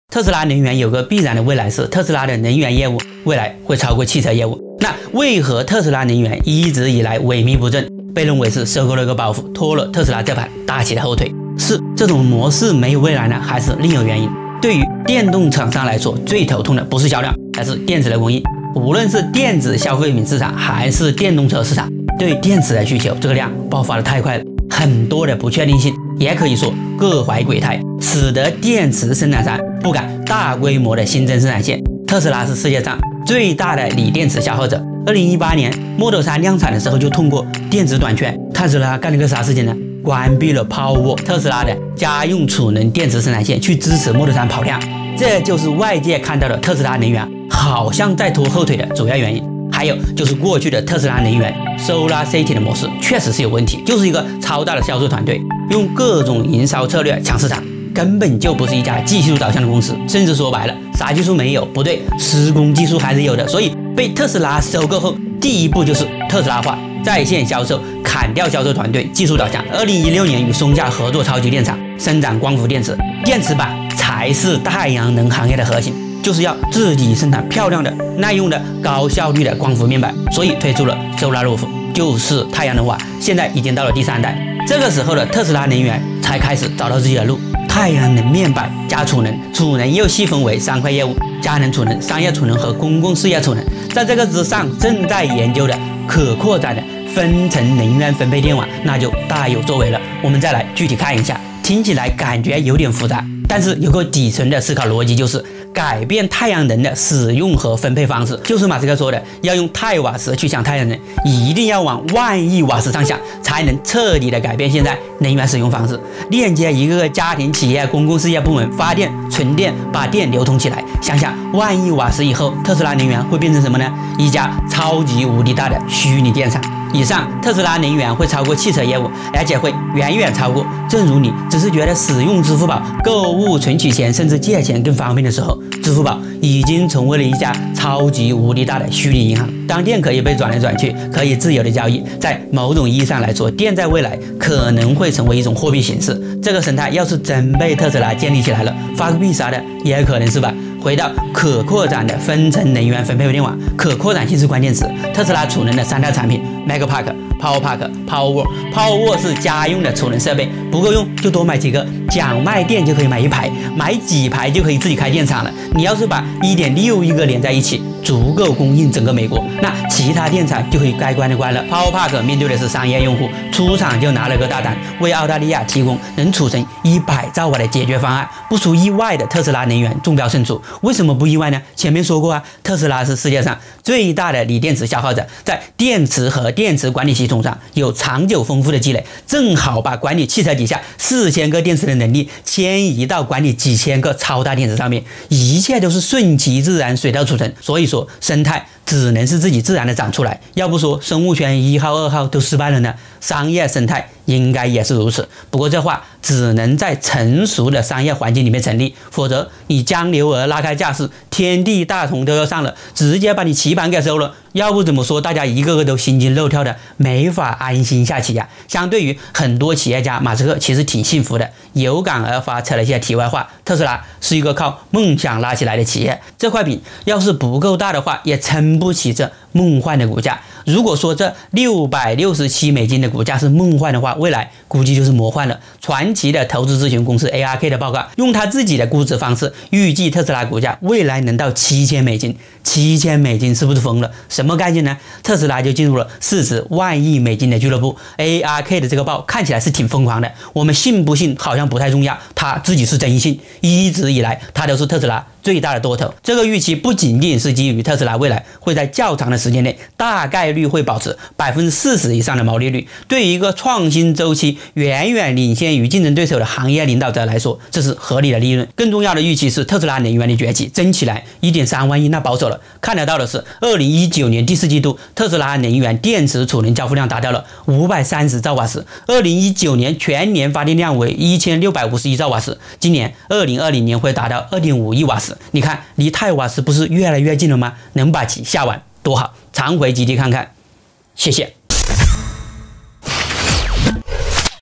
特斯拉能源这个包袱，真不行了吗？虚拟电厂会是下一个“怪物”吗？【常回基地看看】
特斯拉能源会超过汽车业务
而且会远远超过
为什么这么说呢？